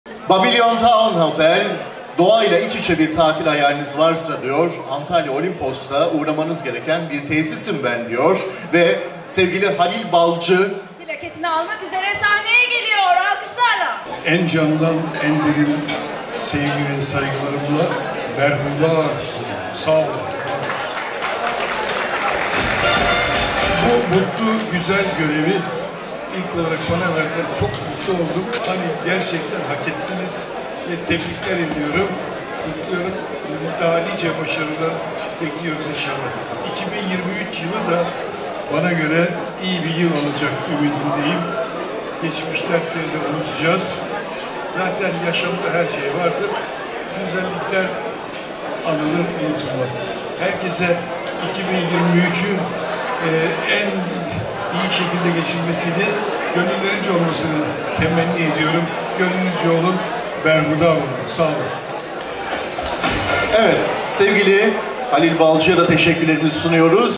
Babylon Town Otel'in teşekkür plaketini Halil Balcı, usta sanatçı Orhan Gencebay'ın elinden aldı.
32 oda 76 yatak kapasitesiyle yeşillikler içerisinde yarım pansiyon olarak hizmet veren Olimpos’taki Babylon Town Hotel, huzur ve sakinlik arayanların gözde adresi.

Gecemizin ana sponsorlarından birisi olan Babylon Town Otel'in teşekkür plaketini otel kurucusu Halil Balcı, usta sanatçı Orhan Gencebay'ın elinden aldı.